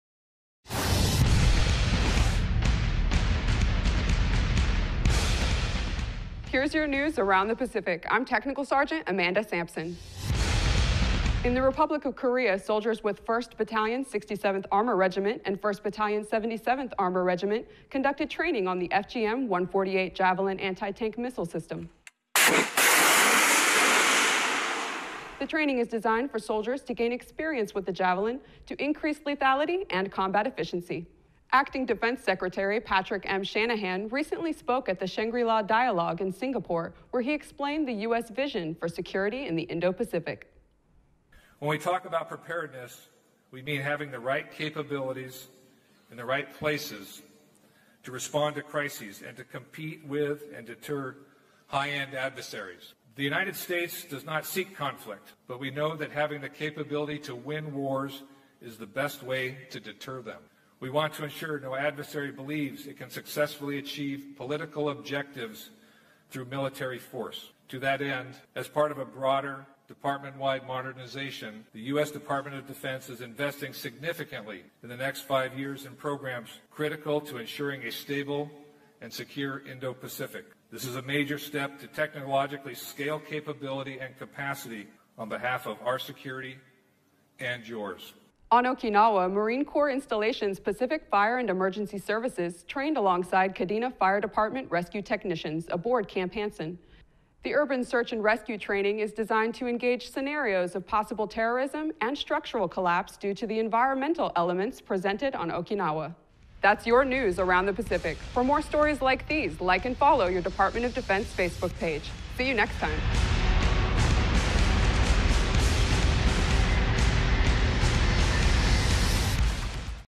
DoD News in 2: June 7, 2019 🇺🇸
🔴 [Author]:
Video by Lance Cpl. Maxwell Gargas

🔴 [Unit]:
Media Center - Japan

🔴 [Location]:
Japan

🔴 [Description]:
On this Ni2, soldiers with the 67th and 77th Armor Regiments conduct training on a javelin anti-tank missile system, Acting Secretary of Defense Patrick M. Shanahan speaks at the Shangri-La Dialogue in Singapore, and fire and rescue technicians train together in Okinawa.

🔴 [Tags]:
Missile, Japan, Emergency Response, SECDEF, U.S. Air Force, U.S. Army, Fire and Rescue, INDOPACOM, Korea United States

🔴 [VIRIN]:
190607-M-CQ097-672